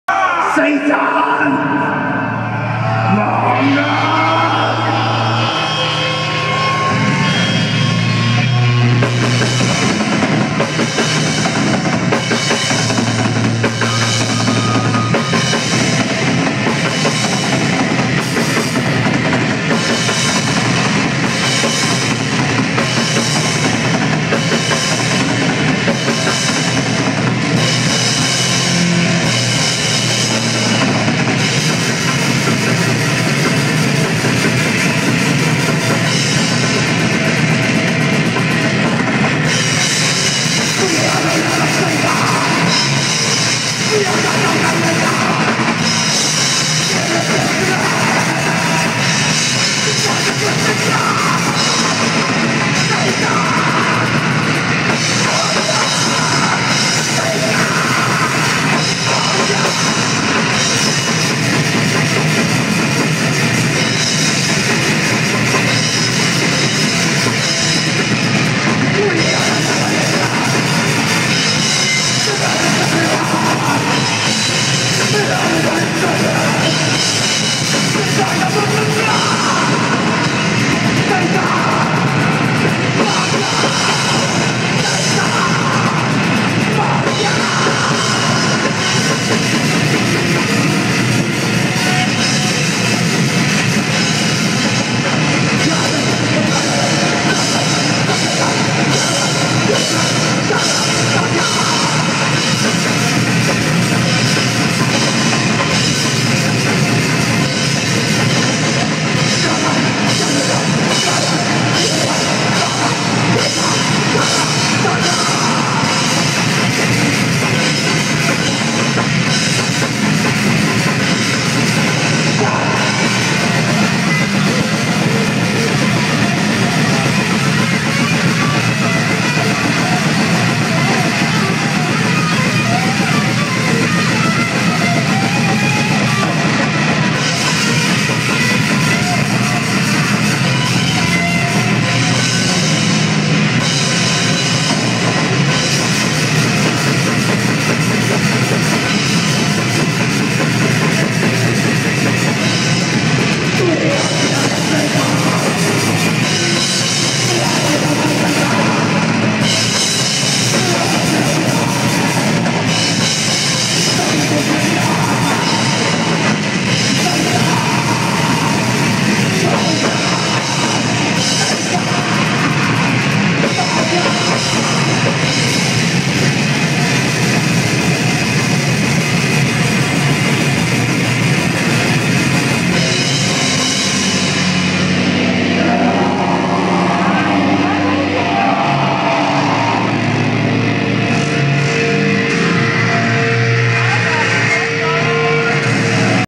Deiphago
Deiphago live in chicago at the cafe lura october 27 2012!!!